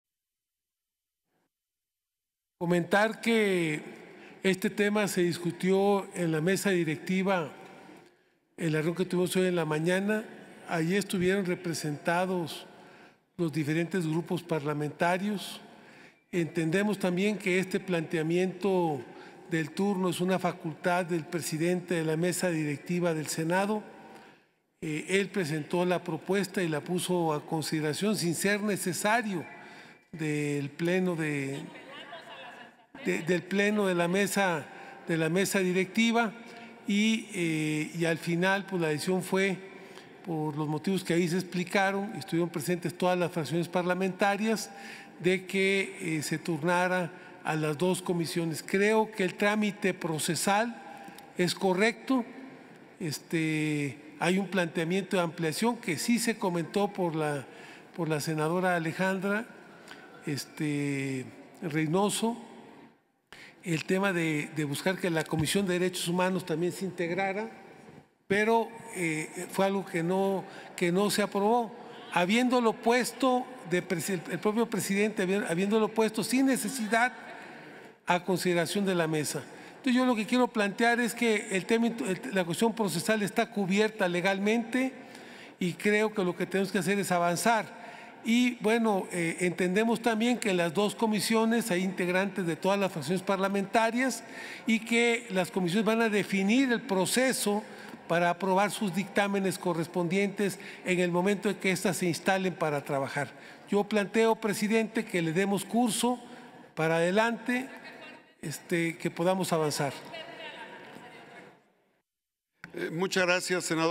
Trámite procesal es correcto: Sen. José Narro (Morena)
El senador José Narro, de Morena, pide avanzar en el trámite a comisiones del proyecto sobre la Guardia Nacional. Precisa que las comisiones definirán el proceso para aprobar los dictámenes correspondientes.